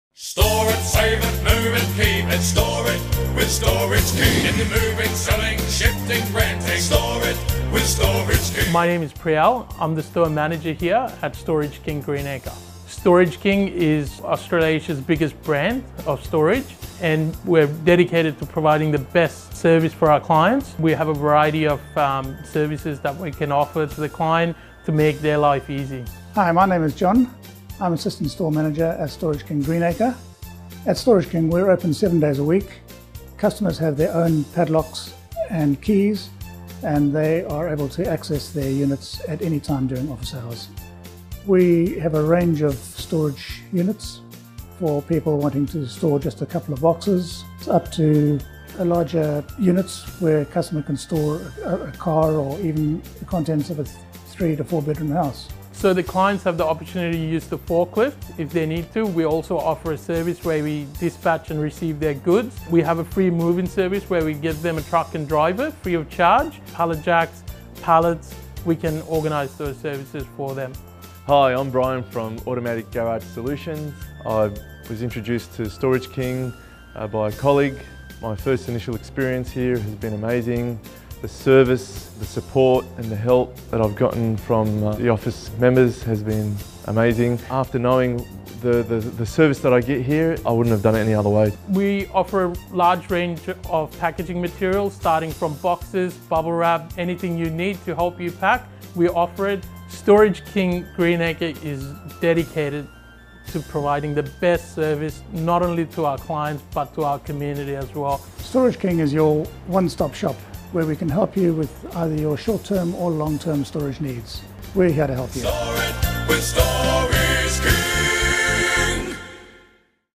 Storage King Greenacre
Meet the team at Storage King Greenacre!

We specialise in Storage for the Greenacre Bankstown Area. At Storage King Greenacre, we have experienced and friendly staff that are ready to assist you with your storage needs. Our large facility conveniently located off the Hume Hwy at Greenacre, (near Bankstown) is purposely built with small and large units available to accommodate your needs.

Storage King Greenacre are proud sponsors of the SES (State Emergency Service) and Birrong Public School.